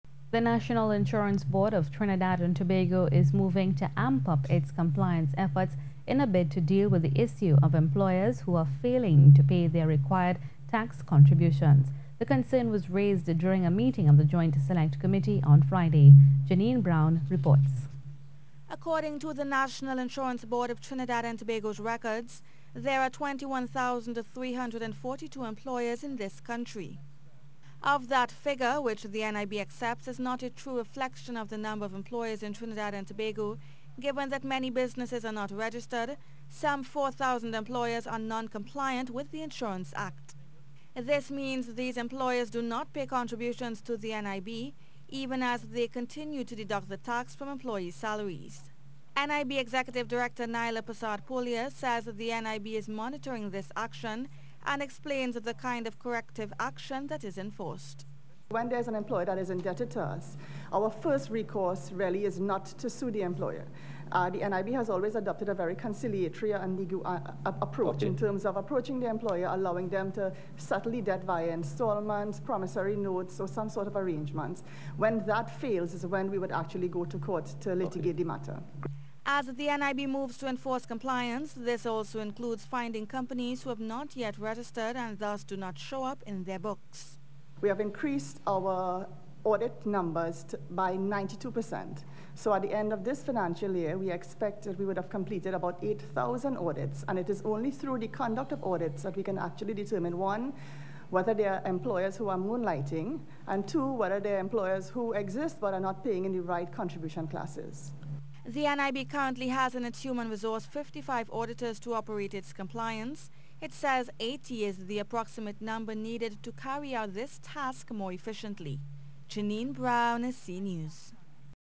NIB To Take Action Against Employers Who Refuse To Pay Contributions For Staff
The National Insurance Board of Trinidad and Tobago is moving to ramp up its compliance efforts, in a bid to deal with the issue of employers who are failing to pay their required tax contributions.